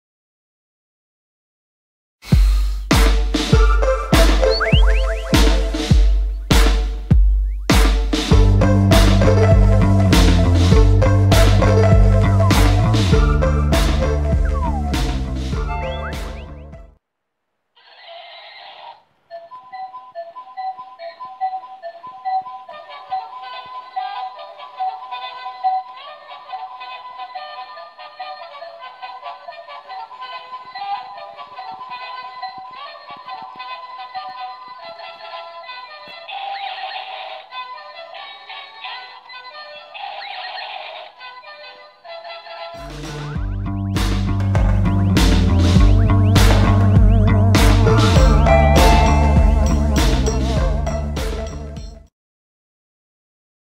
Hrací a chodící koník
Koník který umí hrát a také chodit je jistě vítanou hračkou pro malé děti. Koníka můžou děti vodit a sami pohodlně určovat a korigovat kam půjde. Více informací zde